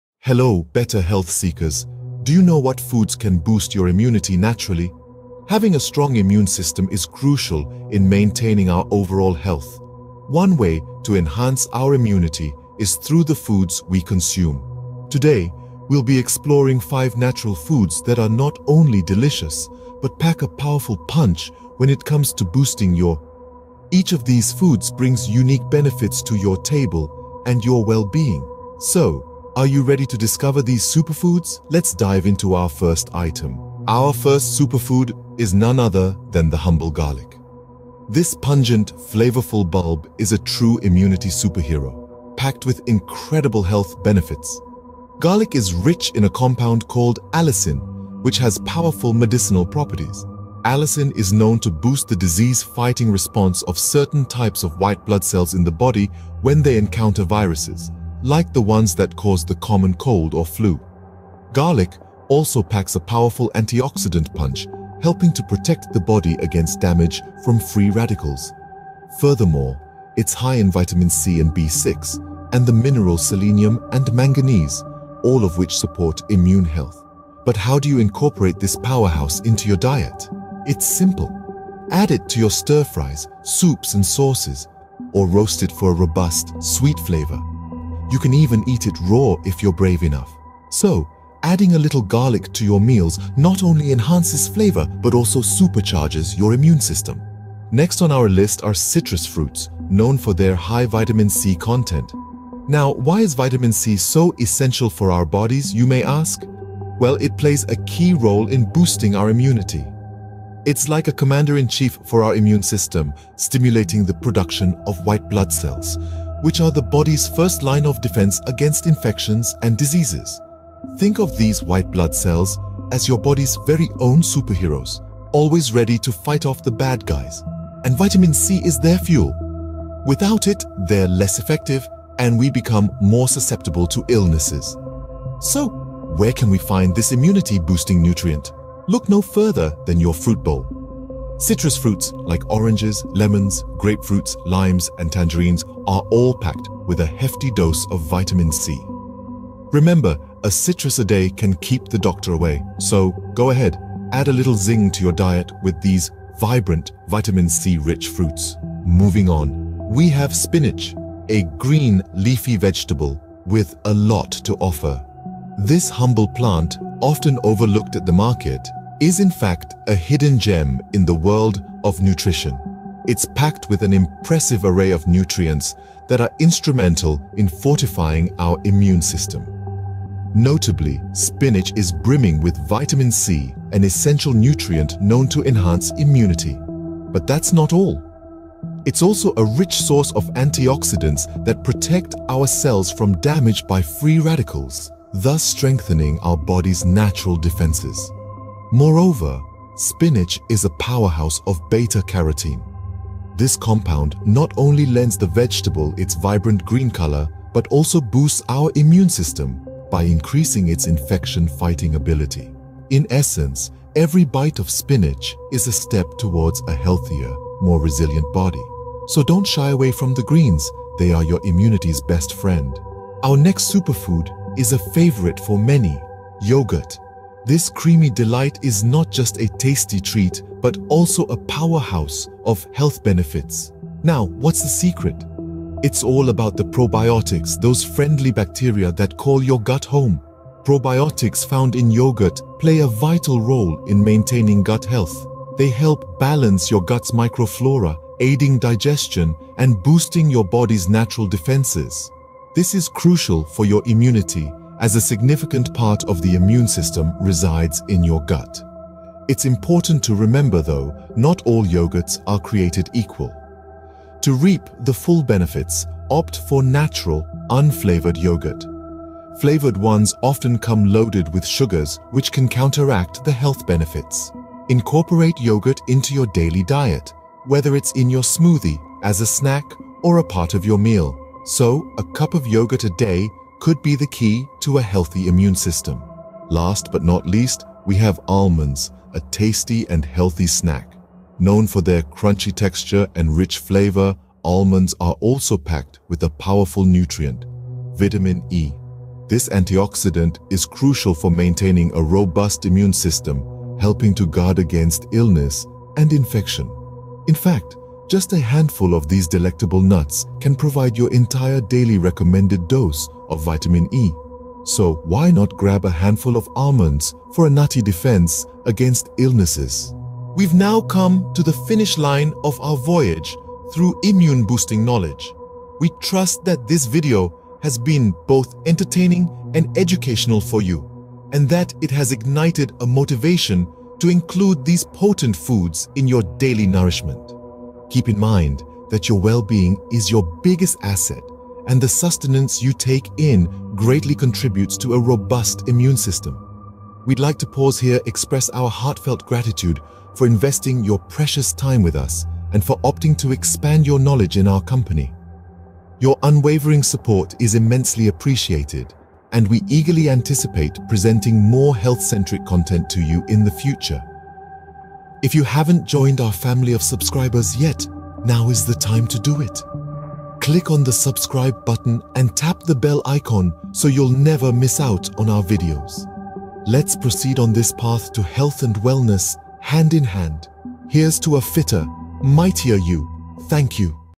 How to boost your immune system FAST! Must watch
Boost your immune system with nature's superheroes! In this video, we explore the top immunity-boosting foods, from garlic to citrus fruits and spinach. Discover how yogurt and almonds can cultivate good health, while also embracing the power of gratitude for a healthier future. Watch now to learn how to defend your body naturally! howto how to boost your immune system fast